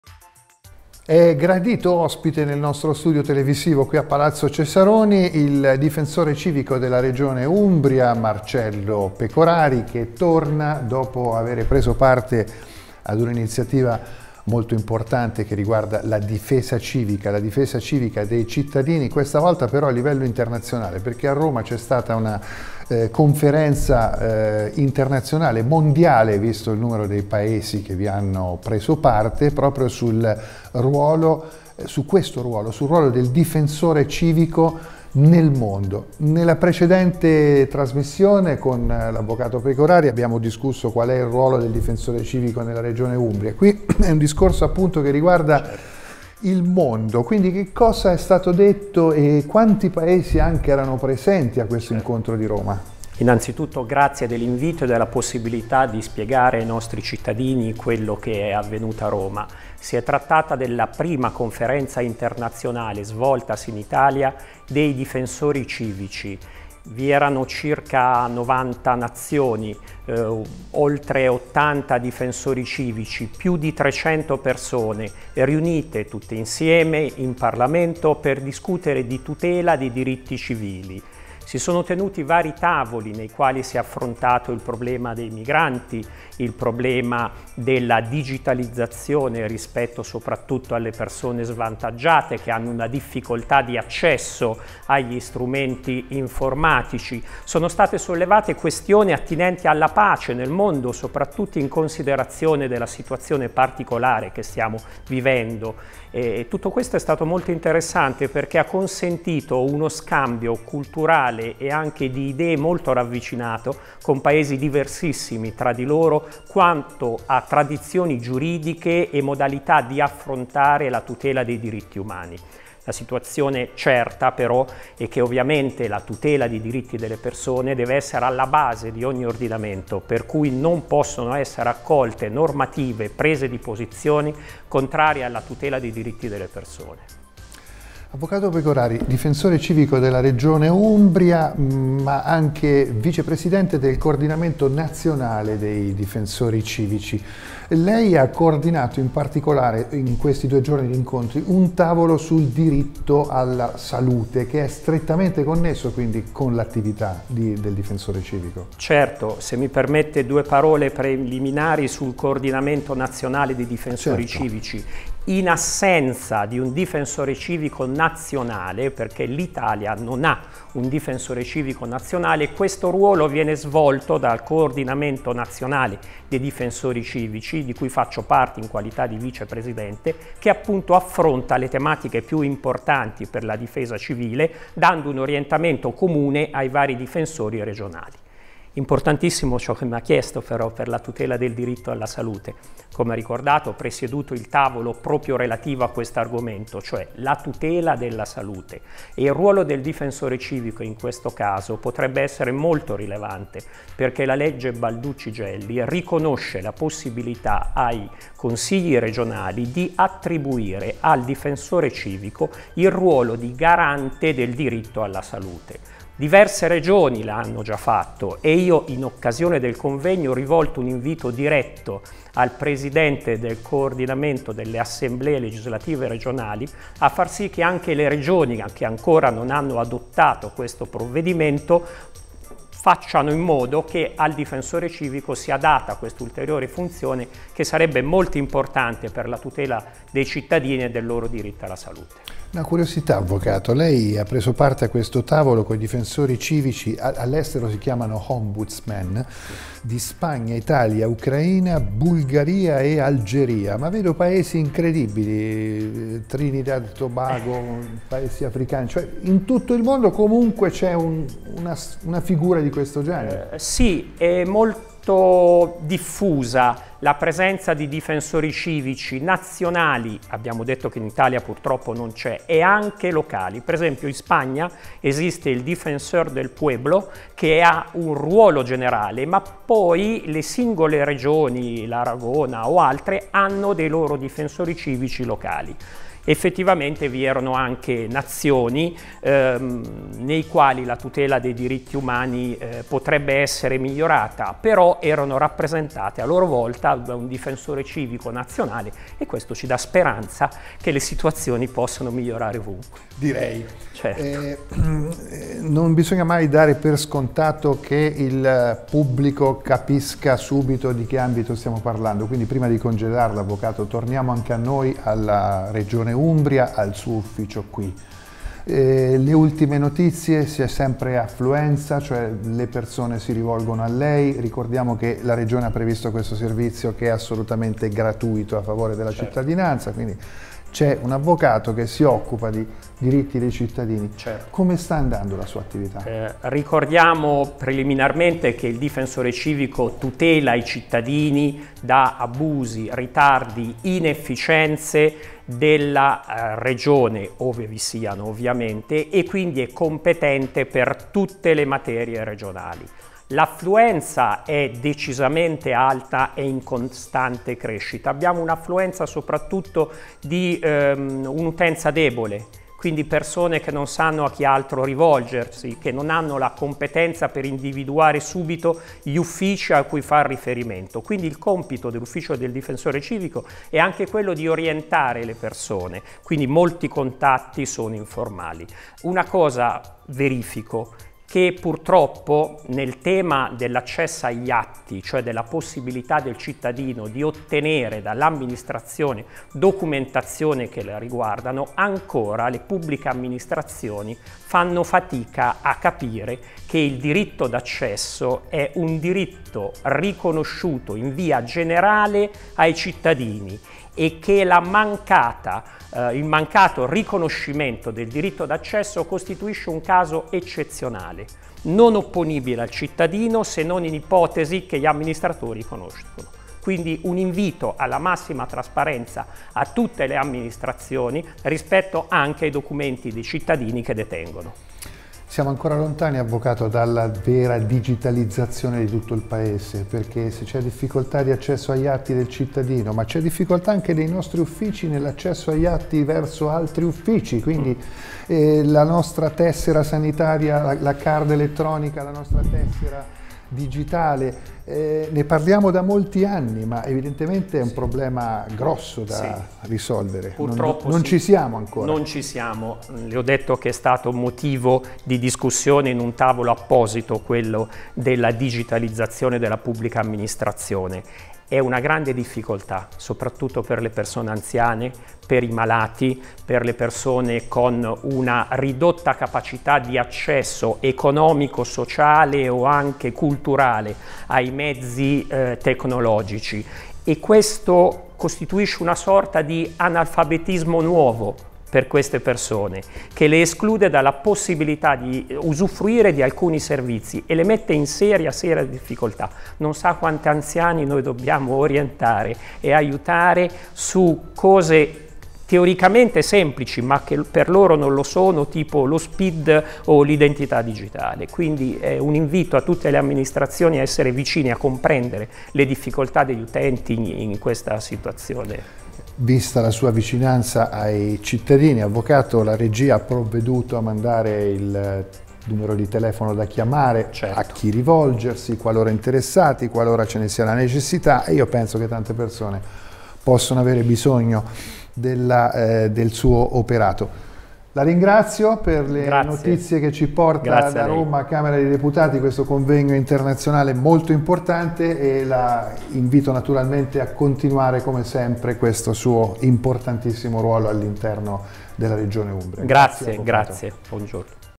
Speciale TeleCRU503 Convegno internazionale Difesa civica HD
Convegno internazionale degli Ombudsman di tutto il mondo. Per l'Umbria il difensore civico della Regione, Marcello Pecorari, anche vicepresidente del Coordinamento nazionale dei difensori civici - intervista